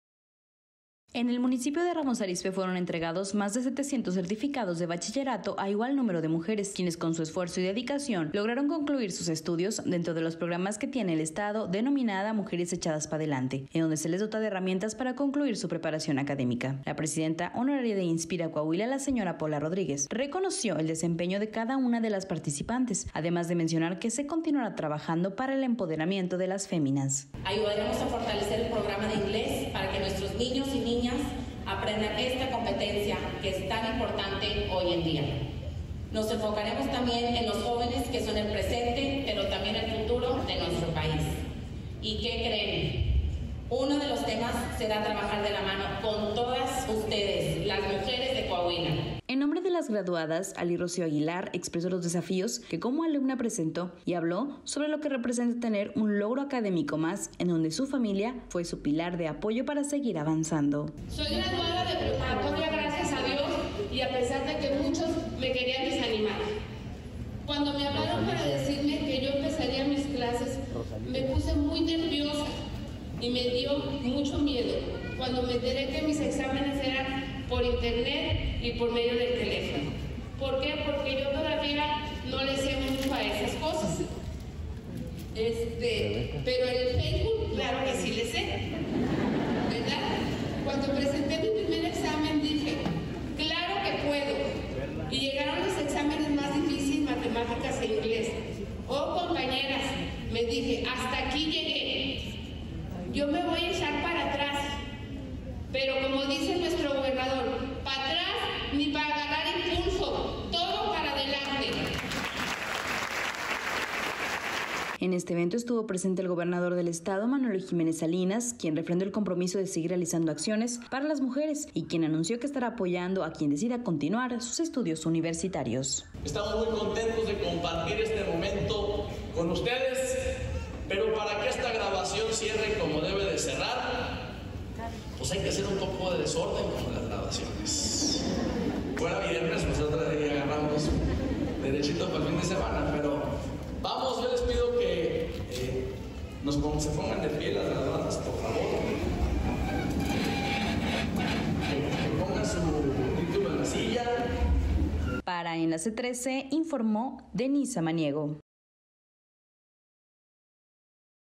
Entregan certificados de preparatoria a más de 700 mujeres
Coahuila | 👩🏻📚 Más de 700 mujeres recibieron su certificado de bachillerato, a través del programa “Mujeres Echadas pa´ delante” en un evento donde el gobernador Manolo Jiménez Salinas refrendó su compromiso de seguir realizando acciones para las mujeres.